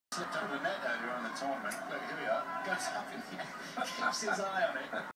Nadal Racquet Catch
He even managed to catch the racquet whilst falling over!